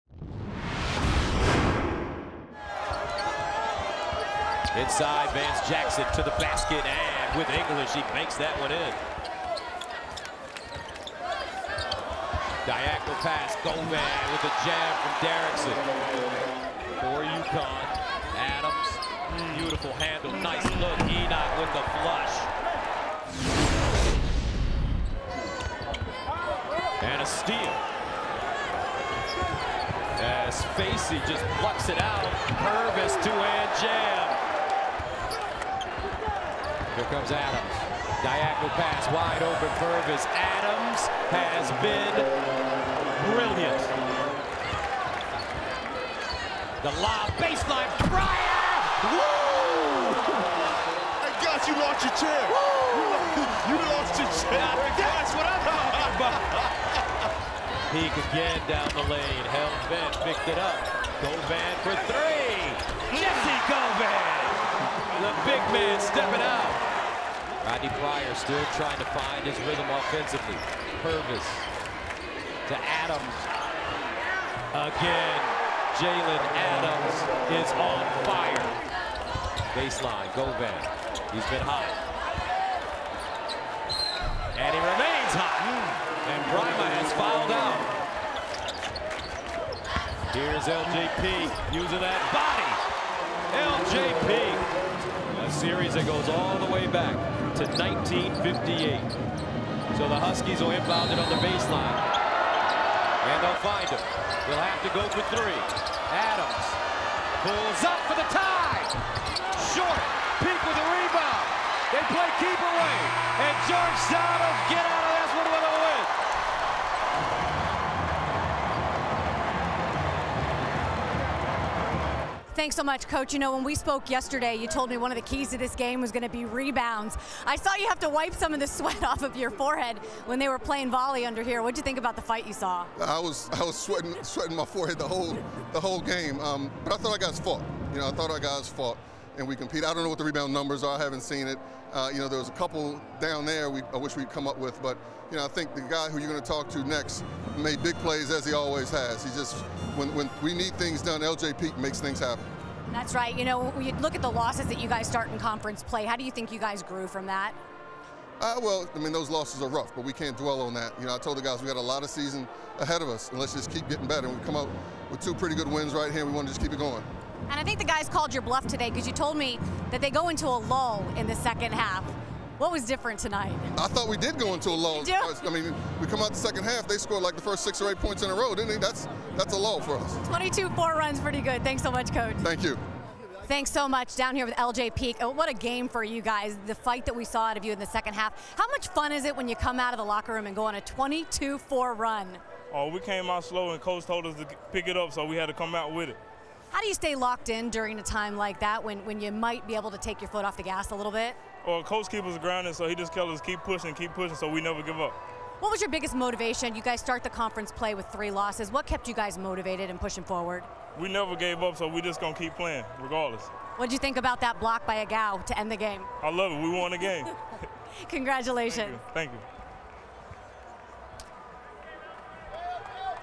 Georgetown rallies UConn 72-69 | 2017 COLLEGE BASKETBALL HIGHLIGHTS
Check out highlights of the Gerogetown Hoyas' win over the UConn Huskies.

About FOX Sports:
FS1 original programs include: Undisputed, The Herd, Speak for Yourself, Garbage Time with Katie Nolan and more.